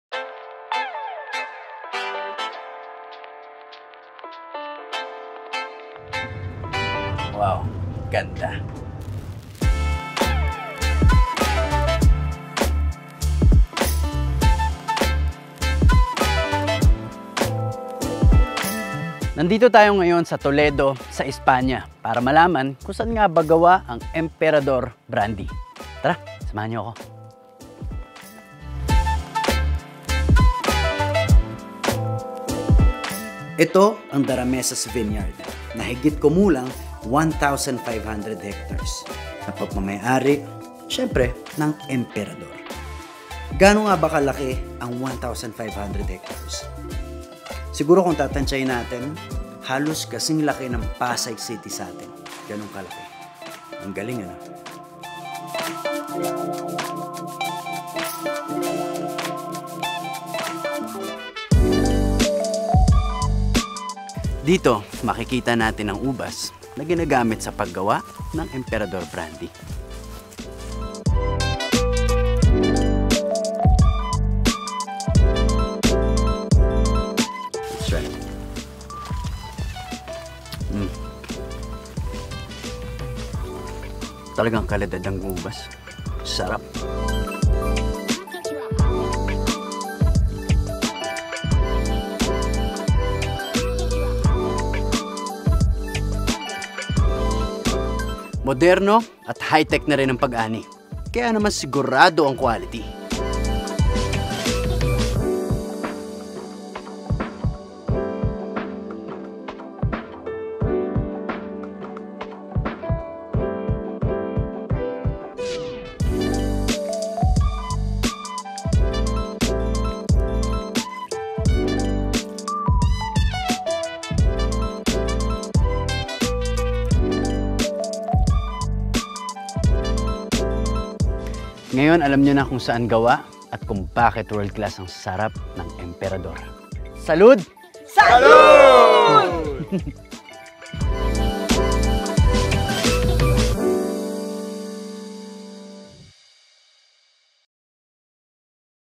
Daniel Padilla visits the Largest Vineyard in Spain
Daramezas Vineyard in Toledo, Spain

This is owned by Megaworld, the Vineyard that produces the
grapes for Emperador.

This is the largest Vineyard, not only in Spain but also in
the whole world.

It covers an area of 1,500 hectares of vineyard land, with around
one million square meters of cellar and bottling facilities, and
four distilleries worldwide.

I am proud to be a Filipino and to see Filipino companies
succeed globally.